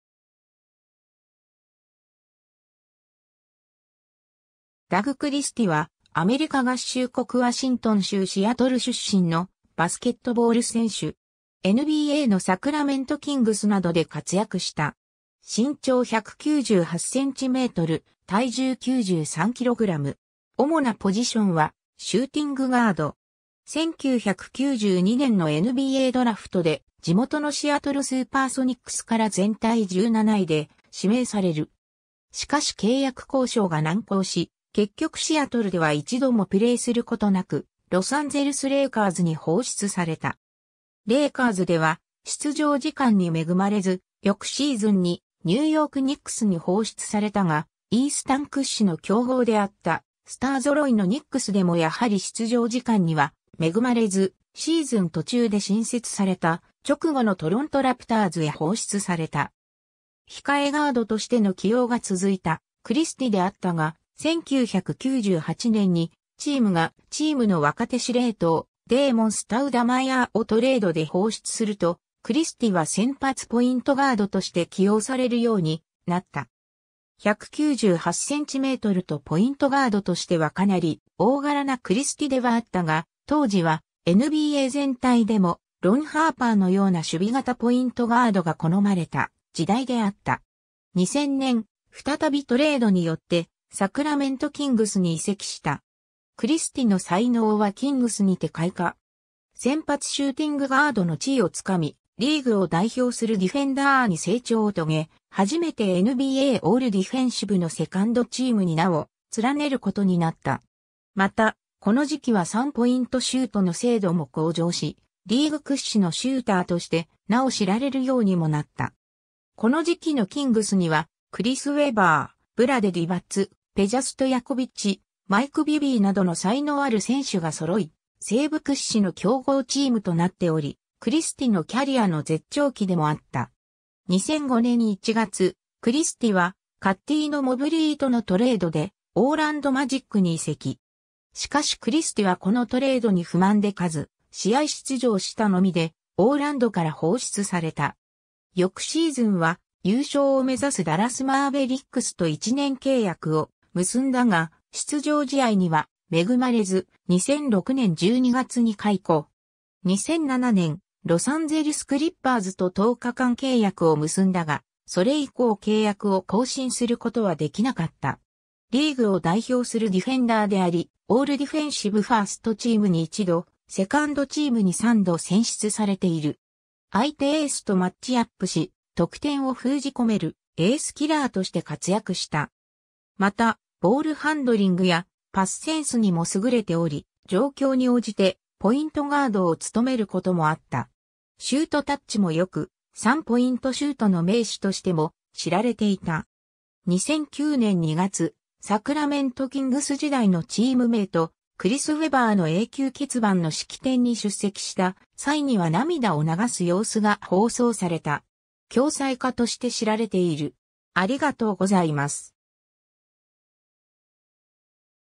ダグ・クリスティ
ダグ・クリスティ（Douglas Dale Christie、1970年5月9日 - ）はアメリカ合衆国ワシントン州シアトル出身のバスケットボール選手。
NBAのサクラメント・キングスなどで活躍した。
主なポジションはシューティングガード。
 1992年のNBAドラフトで地元のシアトル・スーパーソニックスから全体17位で指名される。
しかし契約交渉が難航し、結局シアトルでは一度もプレーすることなくロサンゼルス・レイカーズに放出された。
レイカーズでは出場時間に恵まれず、翌シーズンにニューヨーク・ニックスに放出されたが、イースタン屈指の強豪であったスター揃いのニックスでもやはり出場時間には恵まれず、シーズン途中で新設された直後のトロント・ラプターズへ放出された。
 控えガードとしての起用が続いたクリスティであったが、1998年にチームがチームの若手司令塔デーモン・スタウダマイアーをトレードで放出すると、クリスティは先発ポイントガードとして起用されるようになった。
198cmとポイントガードとしてはかなり大柄なクリスティではあったが、当時はNBA全体でもロン・ハーパーのような守備型ポイントガードが好まれた時代であった。
2000年、再びトレードによってサクラメント・キングスに移籍した。
 クリスティの才能はキングスにて開花。
先発シューティングガードの地位を掴み、リーグを代表するディフェンダーに成長を遂げ、初めてNBAオール・ディフェンシブの2ndチームに名を連ねることになった。
また、この時期は3ポイントシュートの精度も向上し、リーグ屈指のシューターとして名を知られるようにもなった。
この時期のキングスにはクリス・ウェバー、ブラデ・ディバッツ、ペジャ・ストヤコビッチ、マイク・ビビーなどの才能ある選手が揃い、西部屈指の強豪チームとなっており、クリスティのキャリアの絶頂期でもあった。
 2005年1月、クリスティはカッティーノ・モブリーとのトレードでオーランド・マジックに移籍。
しかしクリスティはこのトレードに不満で数試合出場したのみでオーランドから放出された。
翌シーズンは優勝を目指すダラス・マーベリックスと1年契約を結んだが、出場試合には恵まれず、2006年12月に解雇。
2007年、ロサンゼルス・クリッパーズと10日間契約を結んだが、それ以降契約を更新することはできなかった。
 リーグを代表するディフェンダーであり、オールディフェンシブ1stチームに1度、2ndチームに3度選出されている。
相手エースとマッチアップし、得点を封じ込めるエースキラーとして活躍した。
また、ボールハンドリングやパスセンスにも優れており、状況に応じてポイントガードを務めることもあった。
シュートタッチも良く、3ポイントシュートの名手としても知られていた。
 2009年2月、サクラメント・キングス時代のチームメイト、クリス・ウェバーの永久欠番の式典に出席した際には涙を流す様子が放送された。
 恐妻家として知られている。